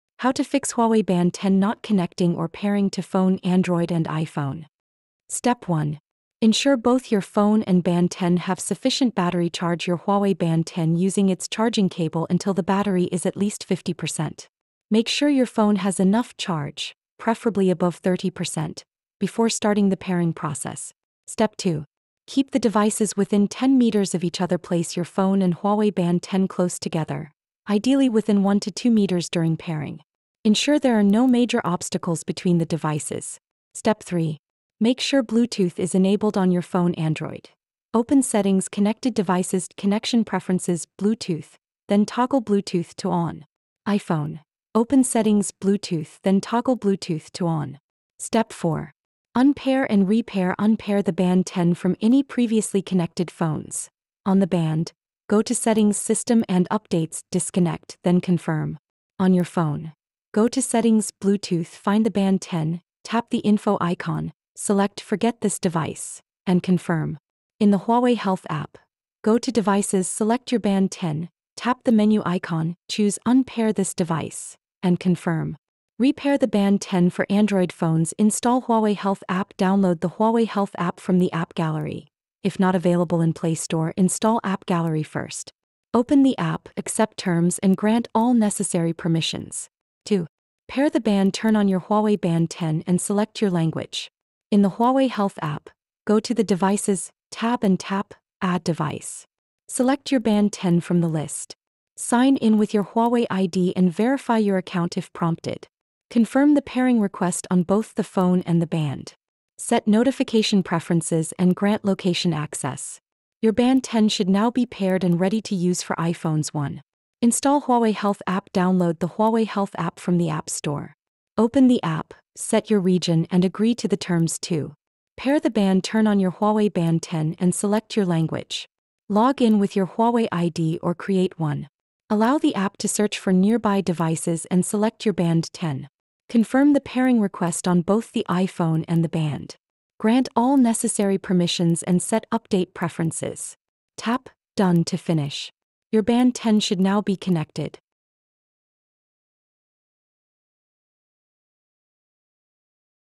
How To Fix Huawei Band 10 Not Connecting Or Pairing To Phone | Android & iPhone
In this comprehensive tutorial, we will guide you through the steps to fix the connectivity issues with your Huawei Band 10. If you're experiencing problems connecting or pairing your device with your Android or iPhone, this video is for you. We will cover common troubleshooting methods, including resetting your device, checking Bluetooth settings, and ensuring compatibility with your smartphone.